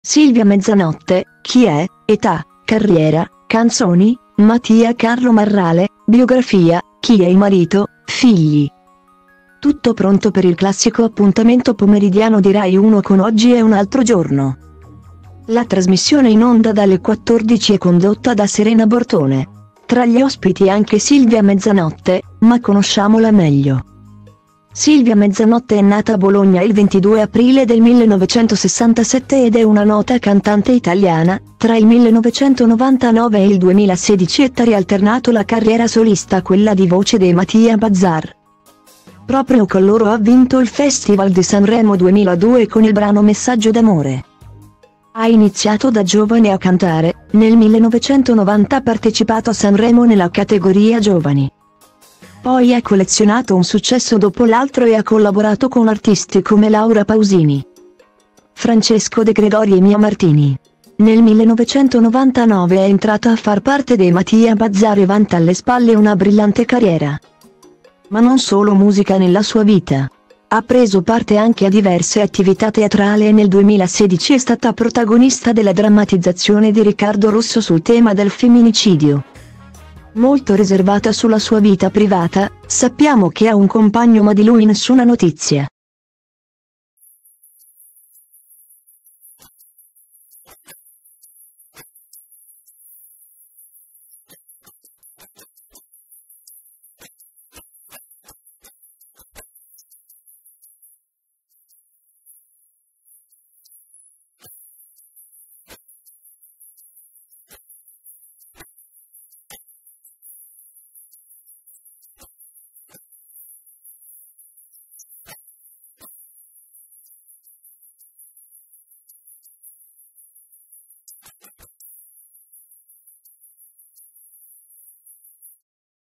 Silvia Mezzanotte: chi è, età, carriera, canzoni, Matia Carlo Marrale, biografia, chi è il marito,
Silvia Mezzanotte: chi è, età, carriera, canzoni, Matia  Carlo Marrale, biografia, chi è il marito, figli
Tutto pronto per il classico appuntamento pomeridiano di Rai 1 con Oggi è un altro giorno,la trasmissione in onda dalle 14 e condotta da Serena Bortone. Tra gli ospiti anche Silvia Mezzanotte, ma conosciamola meglio!Silvia Mezzanotte è nata a Bologna il 22 aprile del 1967 ed è una nota cantante italiana: tra il 1999 e il 2016 ha alternato la carriera solista a quella di voce dei Matia Bazar,